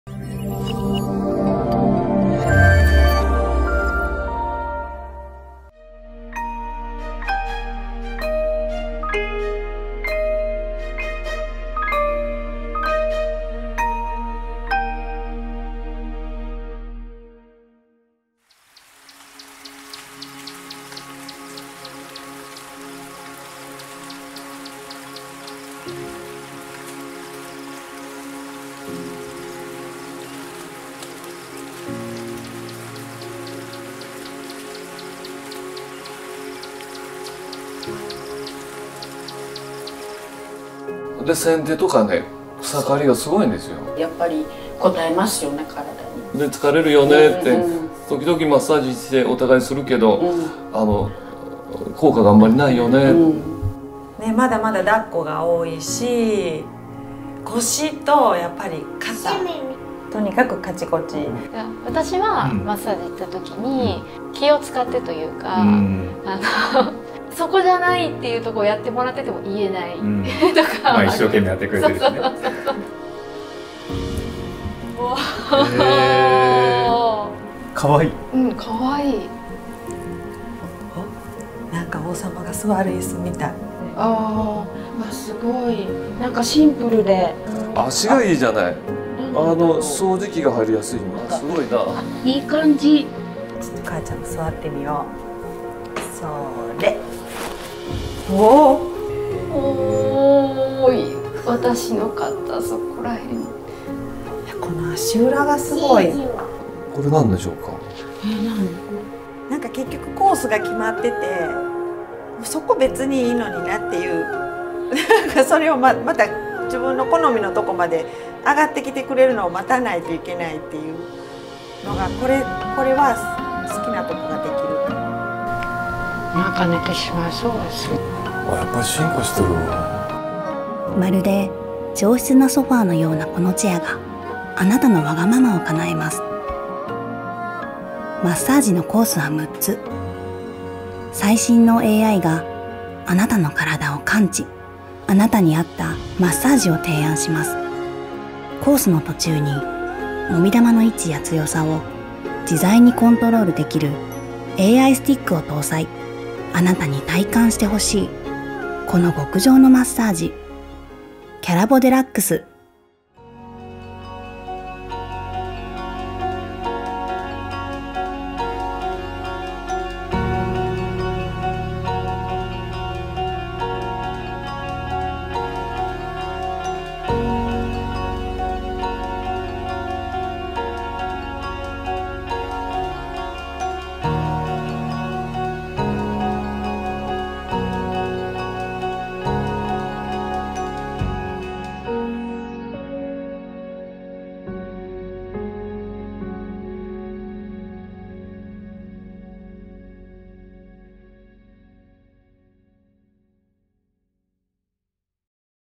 Inada Calabo - Your Home is a Relaxation Salon
• AI Sole Massage Mechanism - Effectively stimulates pressure points of the sole of your feet by pushing in three-dimensional movements.
• AI Stick - It’s like a magical stick, with sensory operations to give you the massage you want
• In addition to the compression effect of conventional air massage, “AI sole mechanism,” a new technology equipped with protrusions that directly stimulate foot pressure points (Yusen and Sokushin), promotes blood circulation from the feet to the whole body.
• Calabo’s Newly Developed Massage Program - Supports a healthy life by offering 6 massage programs with consideration to the best kneading comfort and health maintenance.
• 6 Programs - Full Body Program, Sole Refreshment Program, Neck/Shoulder Program, Program for Stretch for Posture, Defence Body Program, Activity Body Program.
• 3 Programs
o Upper Body - Kneading, Kneading and Tapping, Air
o Lower Body - Sole Kneading, Air
o Roller/Shiatsu -  Full Body, Shiatsu